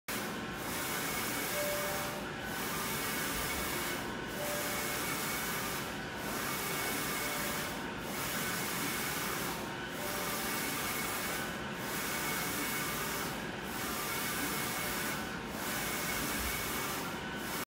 Inkjet printers
Through the machine, the liquid ink is sprayed onto the paper to form a solid state.